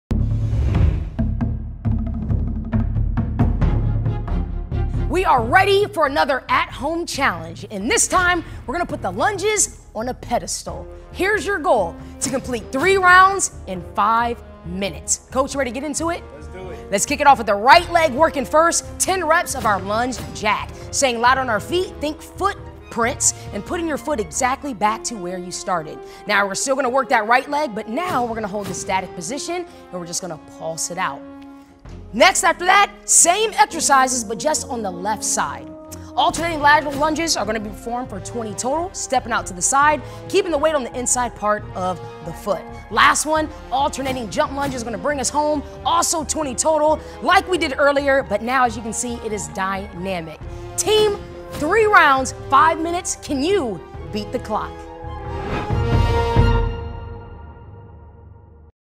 OTFAtHomeChallenge - Lunges!
Time to step up your game with a thigh-burning OrangetheoryAtHome Challenge Edition! It's all about LUNGES!

Tear through just about every type of lunge in the book in just 5 minutes!